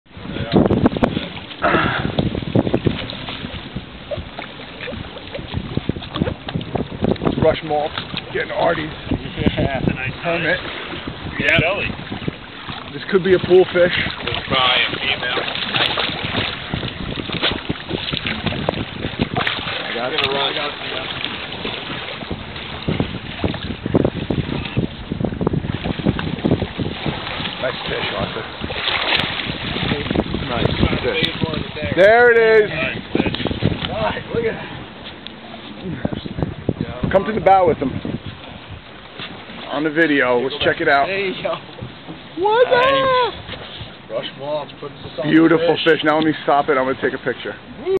Key West Permit Arty V
Arty catches a 20lb permit on 4/18 with Cap Rush Maltz of Oddysea Charters